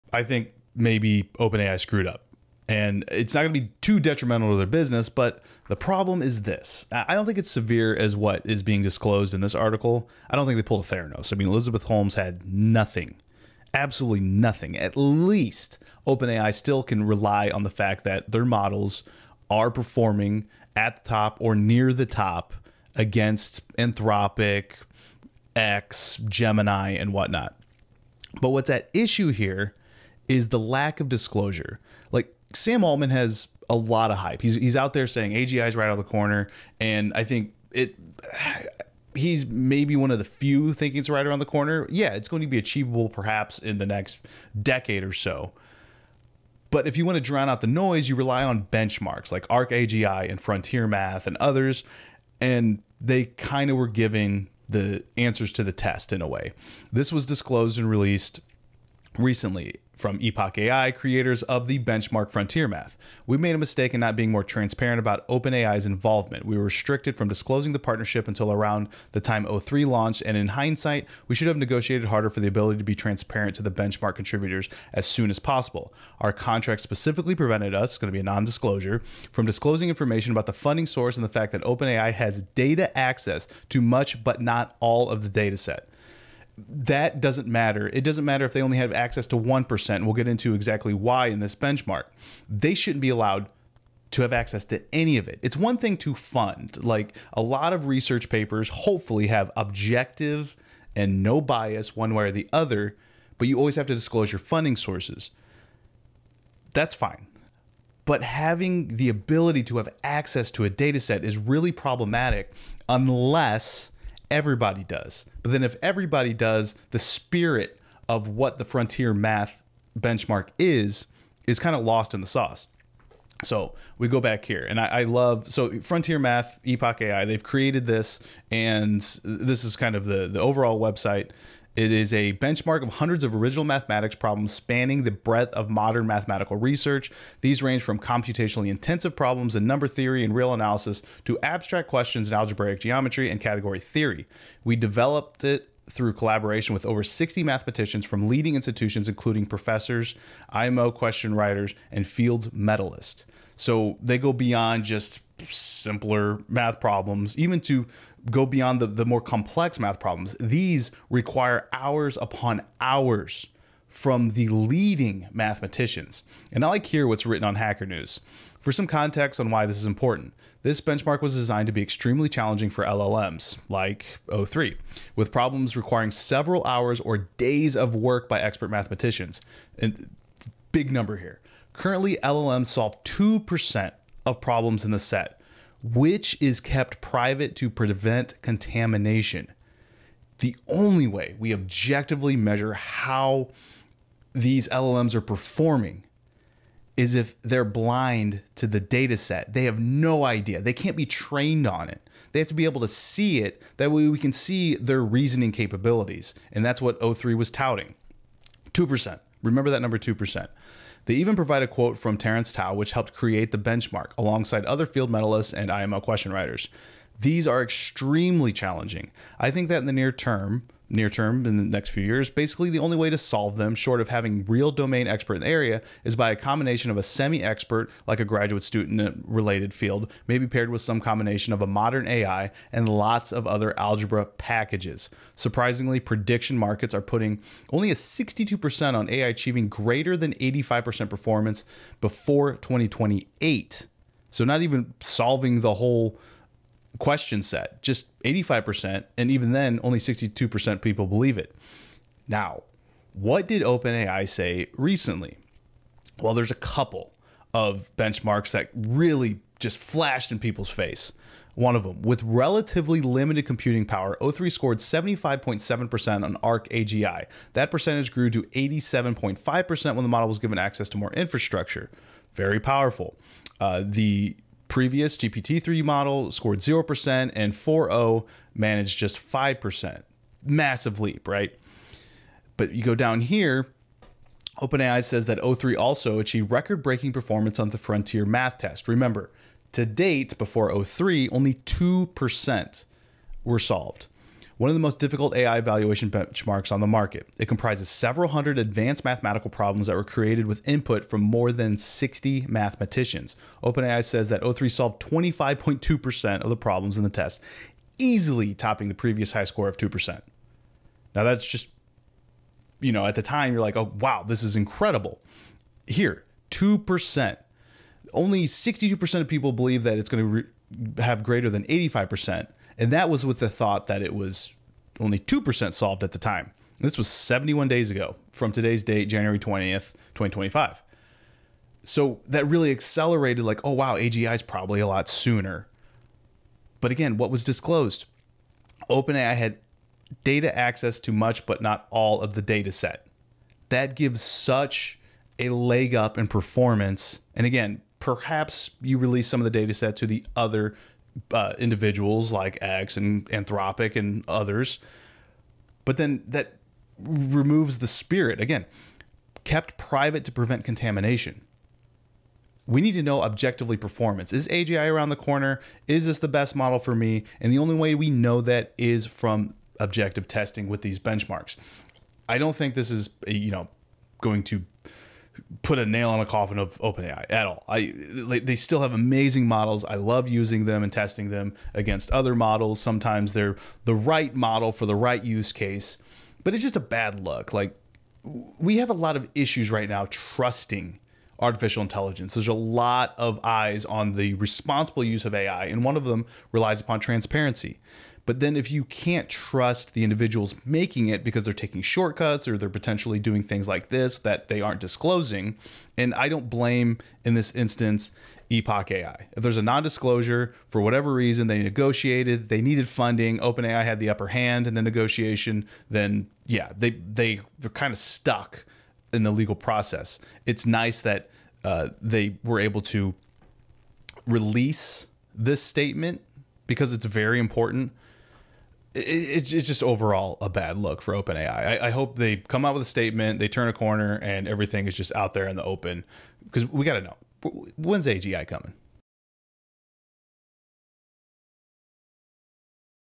Did OpenAI just cheat an IMPORTANT Test?
One of the best ways that we're able to know performance of models, and which one to choose is to look at objective benchmarks. FrontierMath is one of those, and an important one. It involved complex reasoning to resolve problems proposed by top mathematicians. To date, top LLMs have achieved ~2% of resolution, but OpenAI's o3 stated they hit 25%! WOW!

But perhaps there's more to the story?